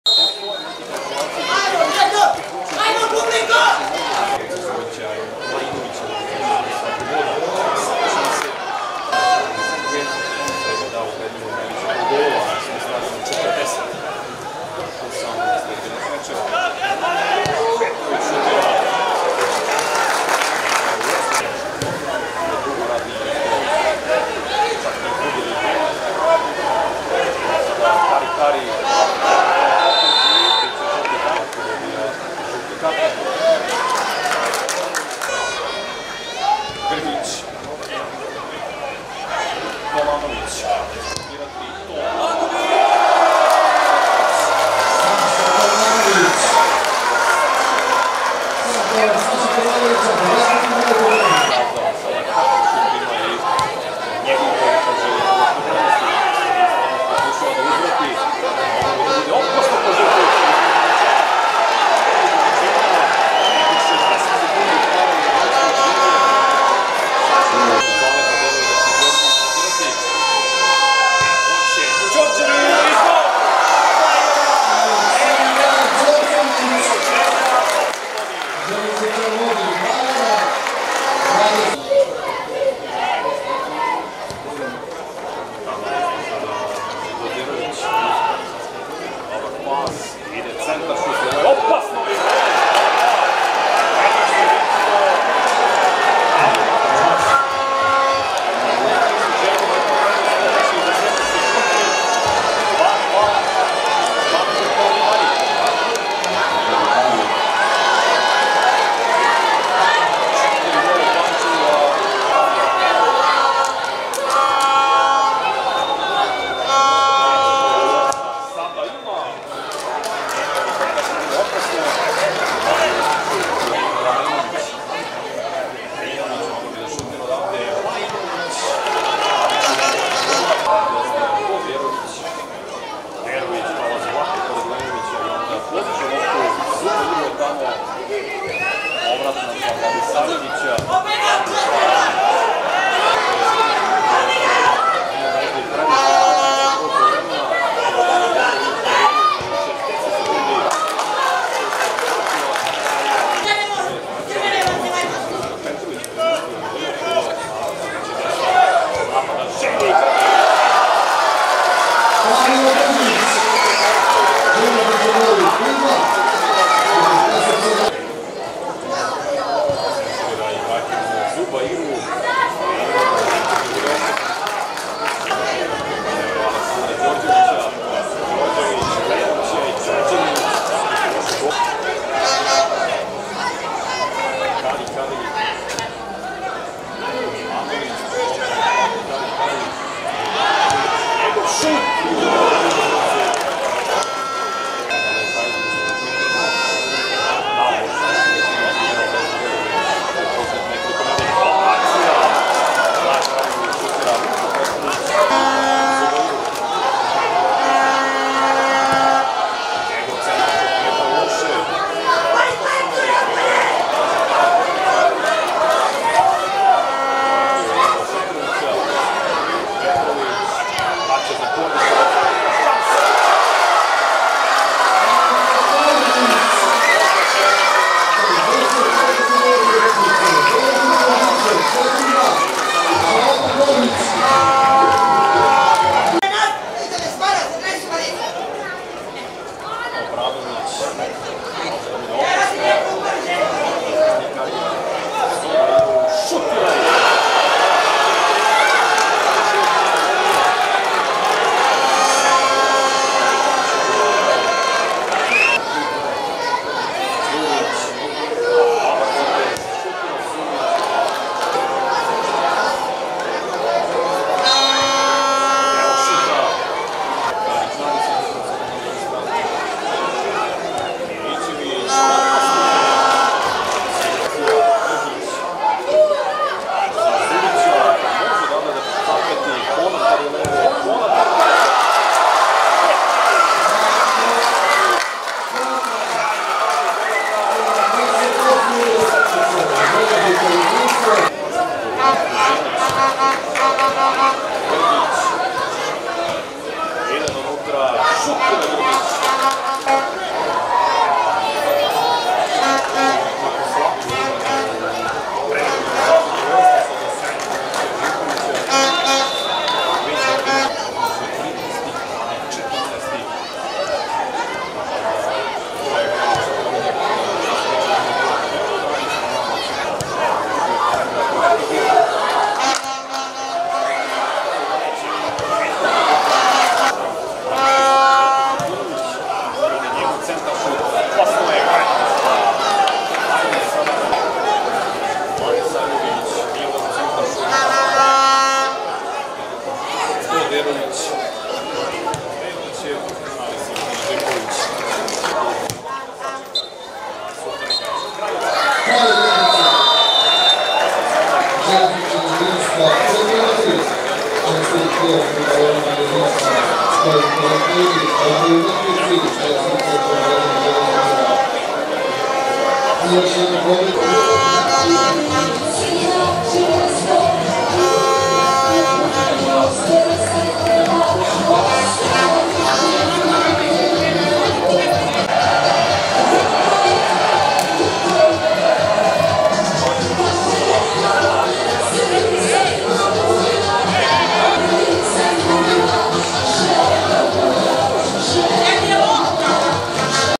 Mozzart Bet Prva liga Srbije 2022/23 - 37.Kolo: ŽELEZNIČAR – JEDINSTVO 5:3 (2:2)
Strelci: Saša Tomanović 19', Aleksandar Đorđević 27', Dario Grgić 47', Šaleta Kordić 69', Marko Konatar 78' - Danilo Miladinović 20', Karikari Kwaku 29', Ibrahim Zubairu 68'